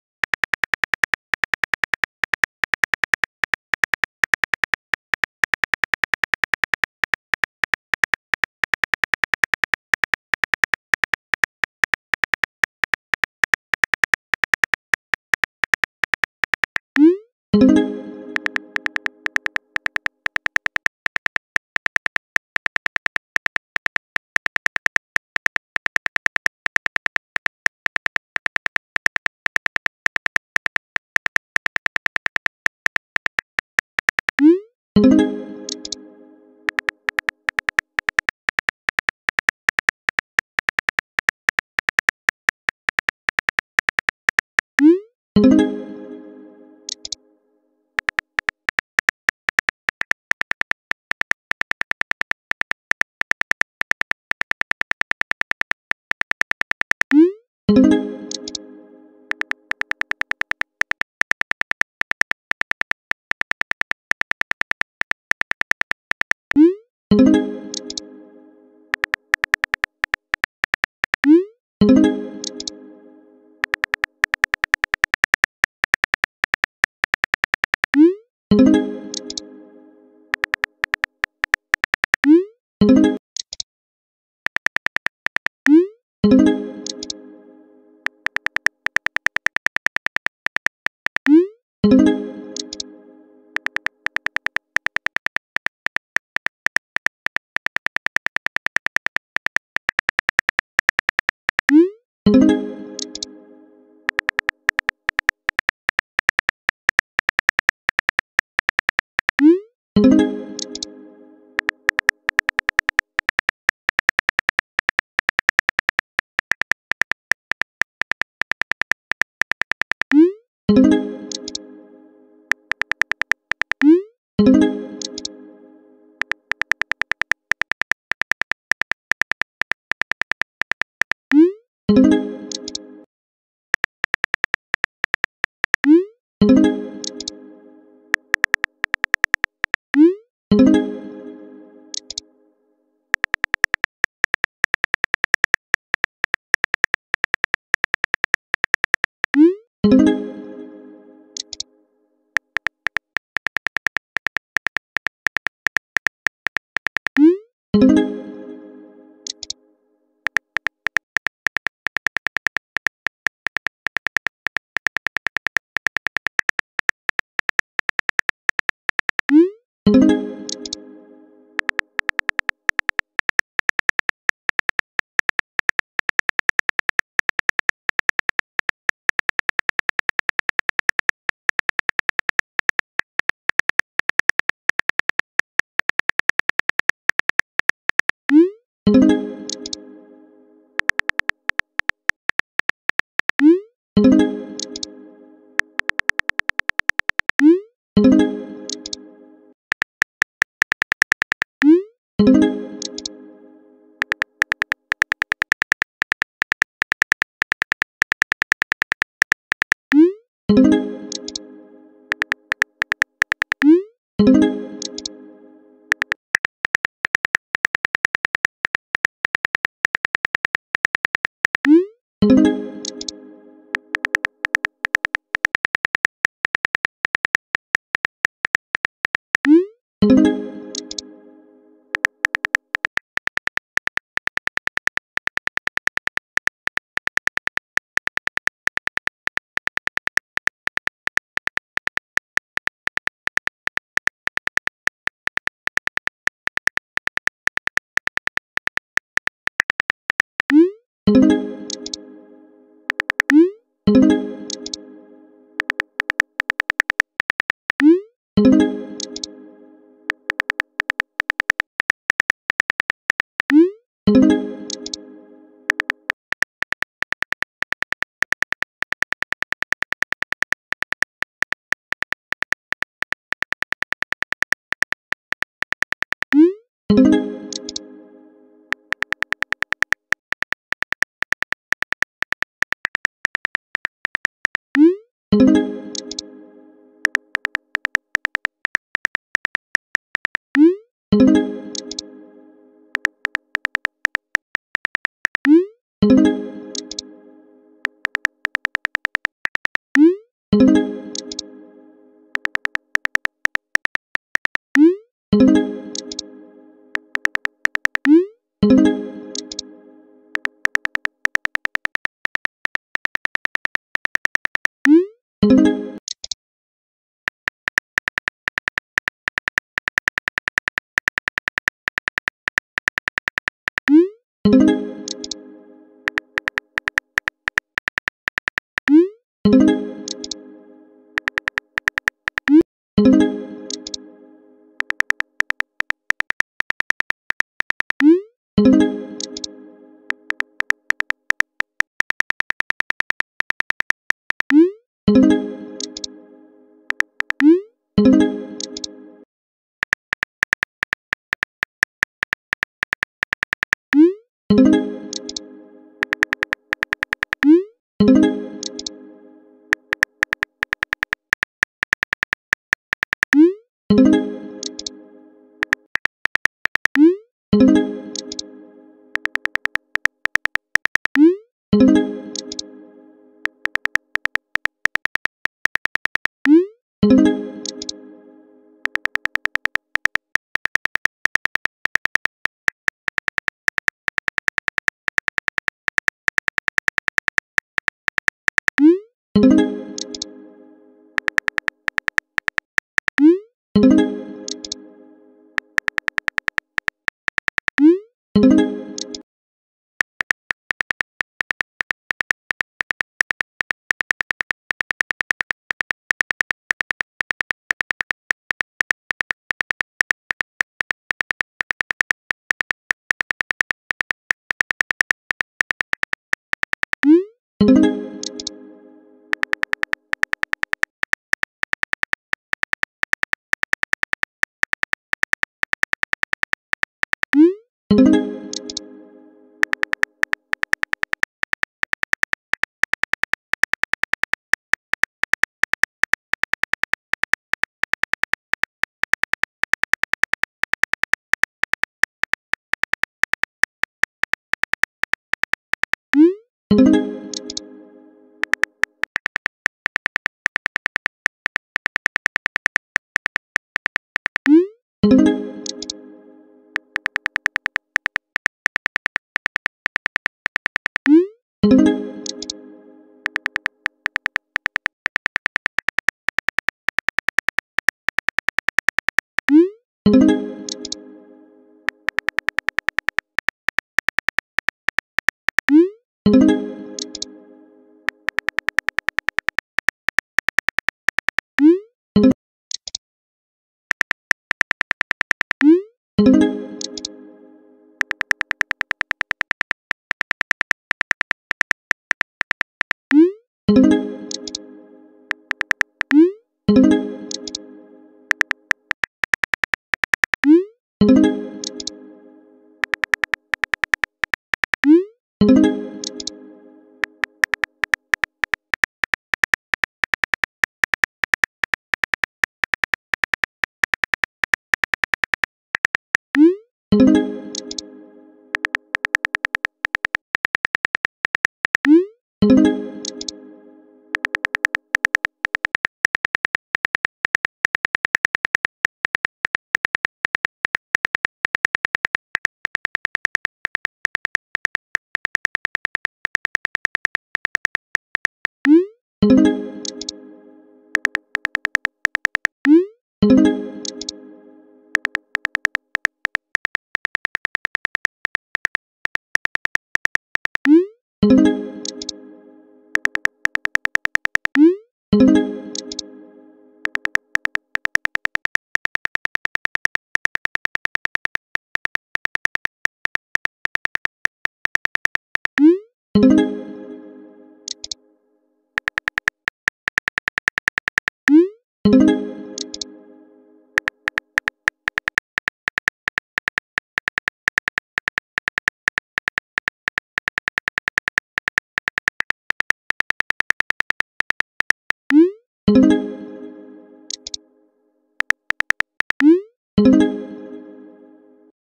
Evie Adopts Carlos’s Baby Part 7: Descendants Texting Story
Hey everyone this is part 7 of my story written by me. When a baby shows up on their door step they’re shocked to find out that this baby is actually Carlos’ baby considering Carlos is only 12. Evie being the good big sister that she is decides to step up and help. Find out what happens next.